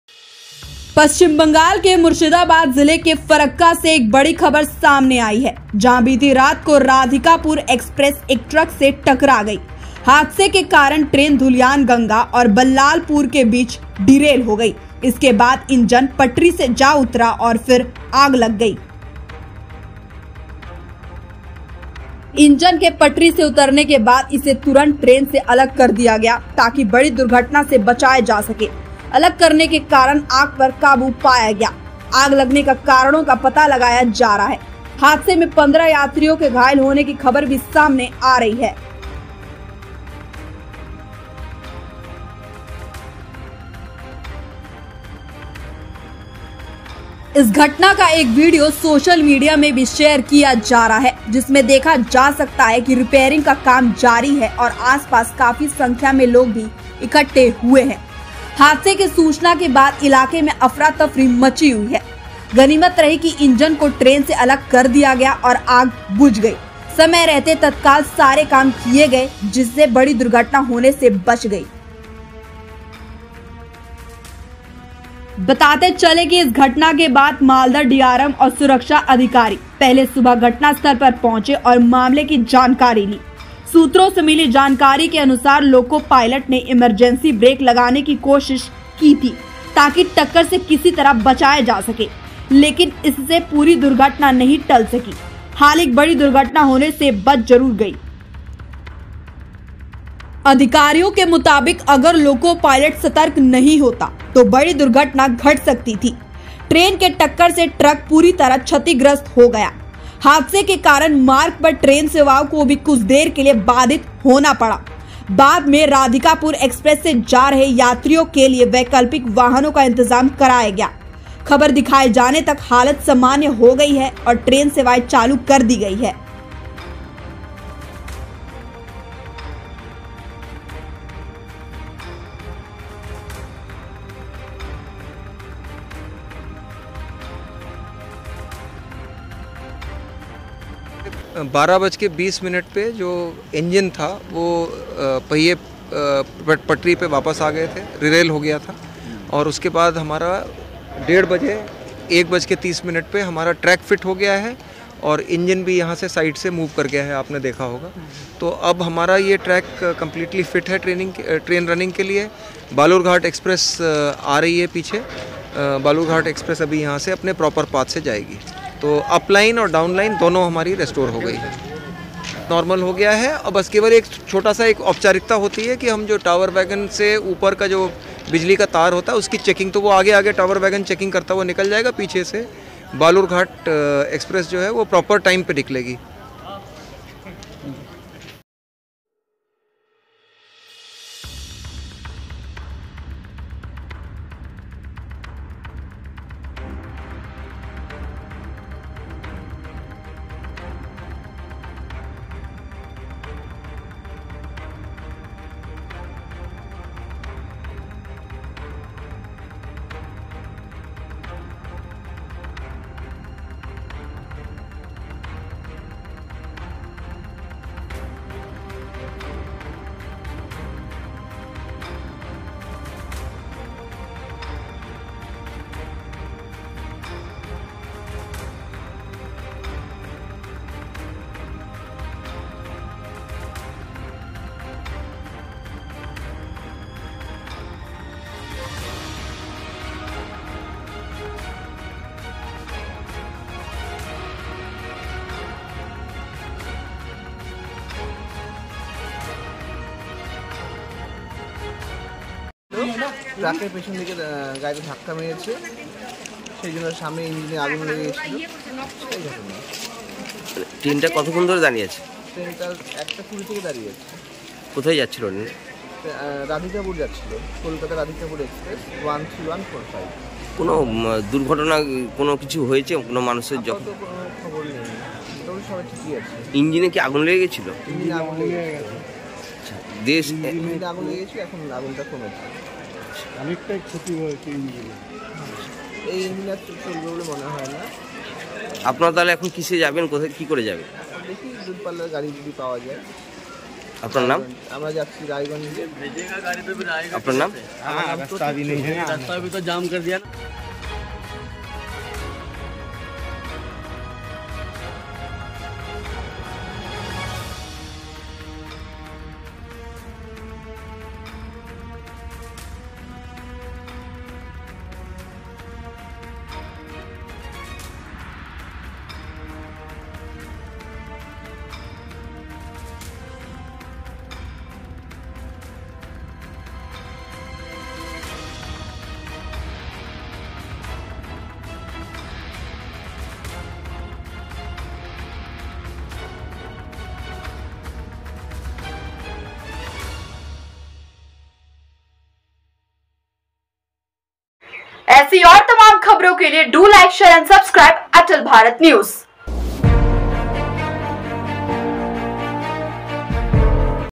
Sahibganj: फरक्का में ट्रेन और ट्रक के बीच जोरदार भिड़ंत में 15 यात्री घायल, मची अफरातफरी, देखें।
Sahibganj: पश्चिम बंगाल के फरक्का में ट्रेन और ट्रक के बीच जोरदार भिड़ंत में 15 यात्री घायल, मची अफरातफरी, देखें पूरी खबर.